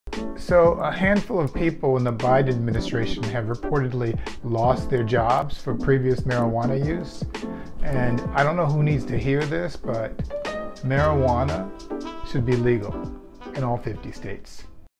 Keith Boykin: Legalize Marijuana
Keith Boykin says the federal government should legalize marijuana.